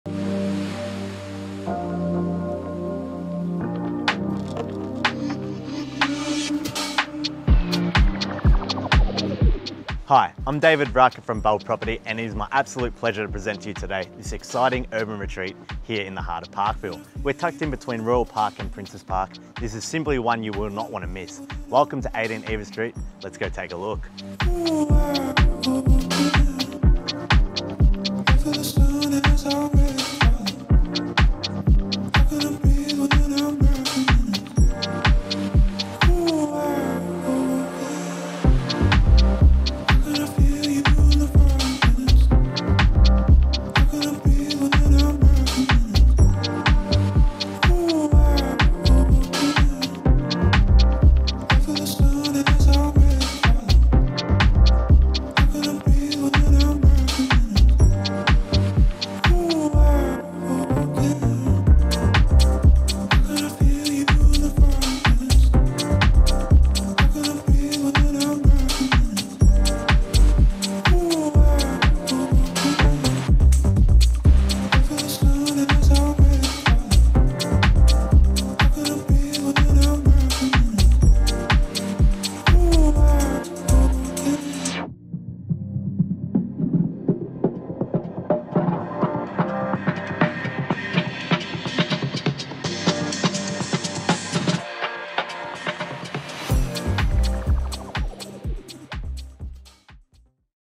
18 Ievers Street, Parkville
Delivering an exciting urban retreat tucked in between Royal Park and Princes Park is this tightly held three-bedroom town home. Spilling over two light-filled levels, beyond the front door lies an inviting haven of charm and easy-care comfort.